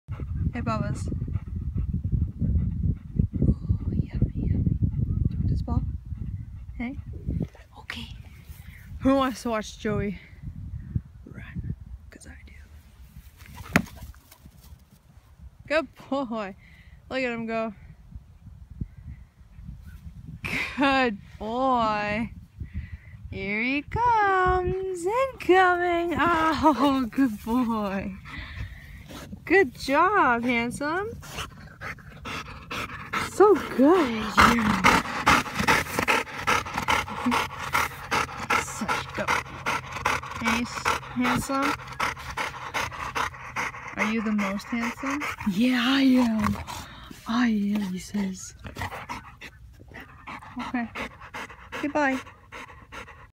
Joey Jax running at the park
Joey Jax playing ball at the park on October 23rd. Fighting lymphoma for 4 months.